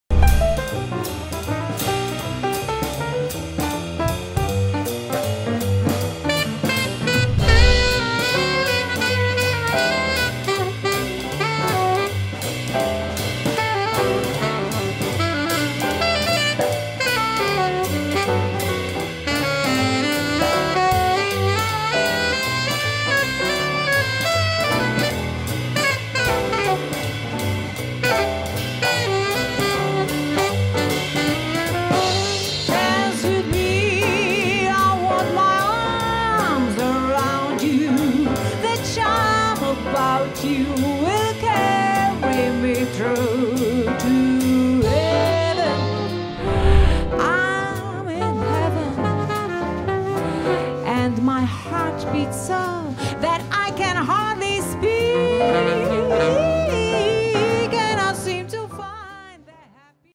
Iris & Ofer Portugaly Featuring Albert Piamenta - promo
Portugaly’s basement TV - טעימה מתוכנית #4, בה נארח את הסקסופוניסט אלברט פיאמנטה, מחלוצי הג'אז בארץ. התוכנית תשודר ב 27/1/21 בשעה 21:00 כניסה לשידור כאן
הצפייה פתוחה לכולם ללא עלות, ואם תרצו, תוכלו לתמוך בפרויקט ובאמנים המדהימים...
Albert Piamenta – alto saxophone
Iris Portugaly – drums & voice
Ofer Portugaly – piano
Eran Zilberberg – bass
Itai Portugaly – sound & mix
Matan Hartman – video & editing